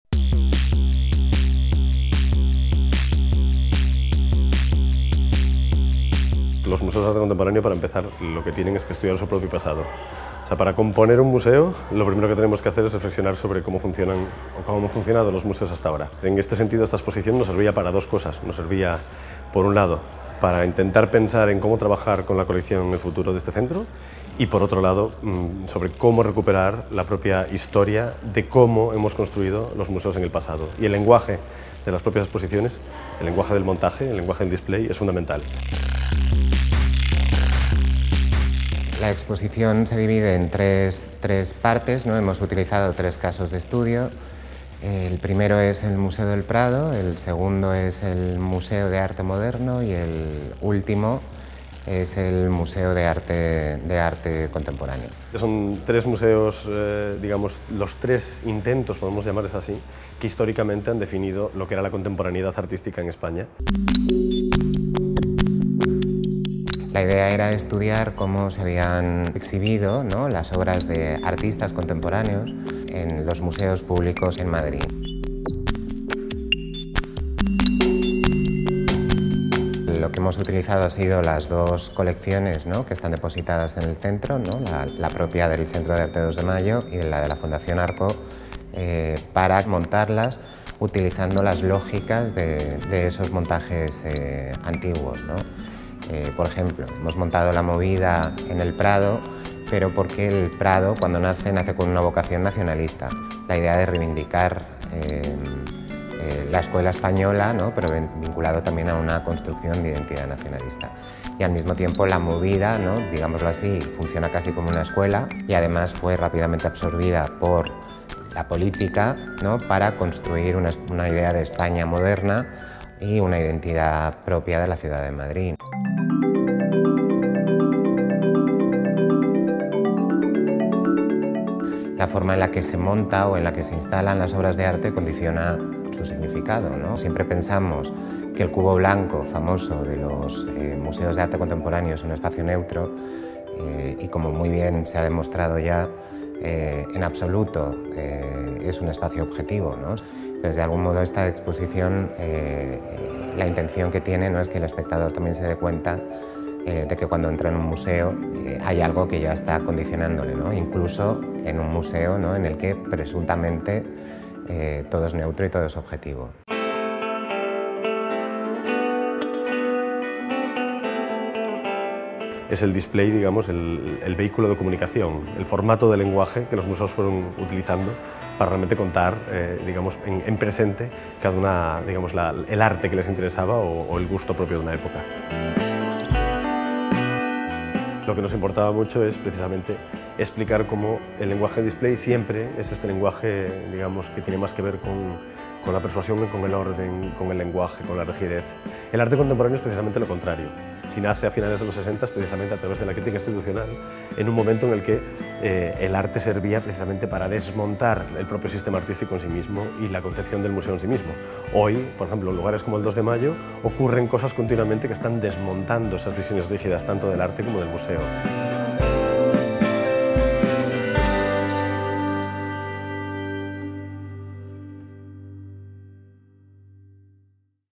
COLECCIÓN XIII. Hacia un nuevo museo de arte contemporáneo. (Exposición).
Colección XIII. Hacia un nuevo museo de arte contemporáneo es una exposición que se plantea como un ensayo de laboratorio y una forma de investigar sobre los dispositivos de exposición que se han utilizado en algunos de los museos públicos vinculados al arte contemporáneo que ha habido en Madrid. Se utilizará la colección del CA2M y de la Fundación ARCO para examinar algunos de estos montajes. No se trata tanto de reconstruirlos, como de probar cómo funcionarían hoy esas formas de montar, intentando evidenciar cómo la percepción y el significado de las obras de arte quedan condicionados por el modo en el que se exhiben. Estos dispositivos traducían la idea que se tenía de arte, exposición y espectador en las épocas en las que se desarrollaron e incluso pueden vincularse a la representación del poder, asociándose al concepto de autoridad, y a la construcción de identidades. Se convirtieron en un elemento que mediaba y modificaba la interpretación de las obras.

Entrevista a Sergio Rubira, comisario de la exposición, y a Manuel Segade, director del CA2M.

Vídeo realizado por: Catorce Comunicación.

COLLECTION XIII. Towards a new museum of contemporary art. (Exposition).

"Collection XIII Towards a new museum of contemporary art" is an exhibition that is presented as a laboratory essay and a way to investigate the exhibition devices that have been used in some of the public museums linked to contemporary art. in Madrid. The collection of the CA2M and the ARCO Foundation will be used to examine some of these assemblies. It is not so much a matter of reconstructing them, as of proving how these forms of riding would work today, trying to show how the perception and meaning of works of art are conditioned by the way in which they are exhibited. These devices translated the idea that was had of art, exhibition and spectator in the epochs in which they were developed and they can even be linked to the representation of power, being associated to the concept of authority, and to the construction of identities. They became an element that mediated and modified the interpretation of the works.

Interview with Sergio Rubira, curator of the exhibition, and Manuel Segade, director of CA2M.

Video made by: Fourteen Communication.